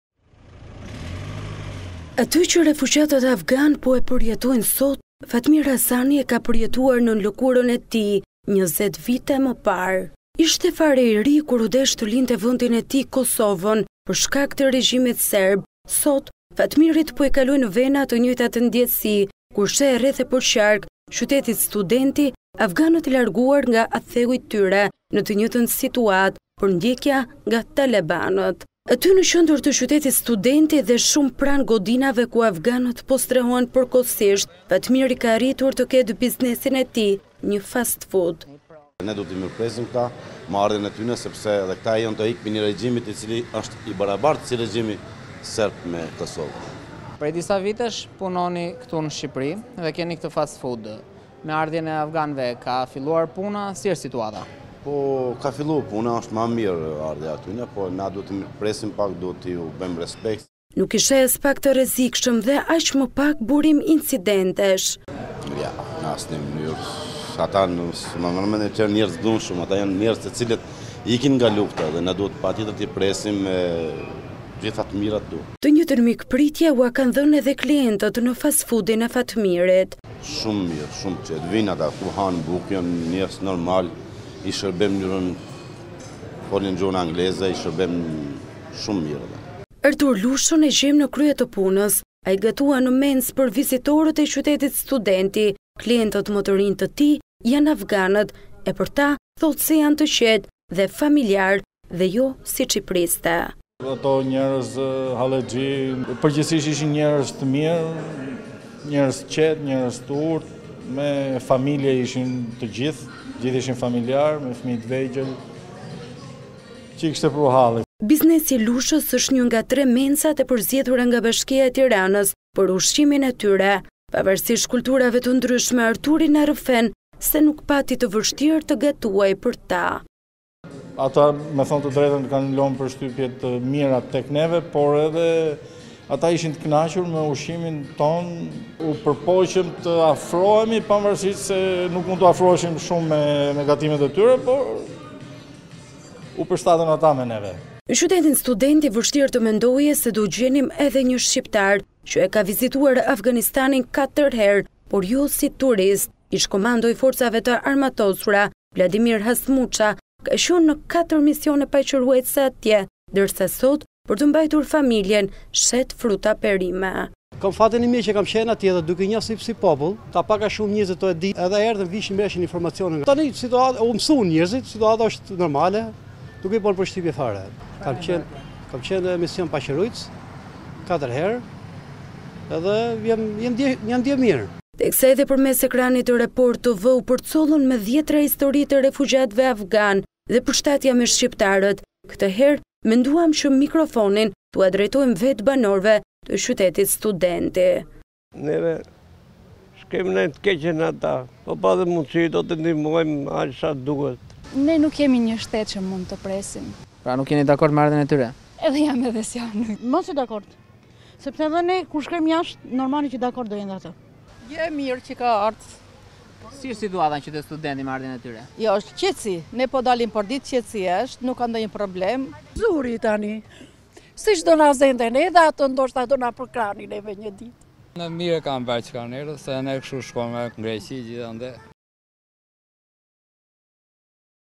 Afganët në sytë e banorëve të 'Qytetit Studenti': Erdhën nga halli, mund të na zinte edhe ne!
TIRANË- Atë që refugjatët afganët po e përjetojnë sot, Fatmir Hasani e ka përjetuar në lëkurën e tij 20 vite më parë.

Ishte fare i ri, ku u desh të linte vendin e tij, Kosovën, për shkak të regjimit serb. Sot, Fatmirit po i kalojnë në vena të njëjtat ndjesi, kur sheh rreth e qark 'Qytetit Studenti', afganët e larguar nga atdheu i tyre në të njëjtën situatë, përndjekja nga talebanët.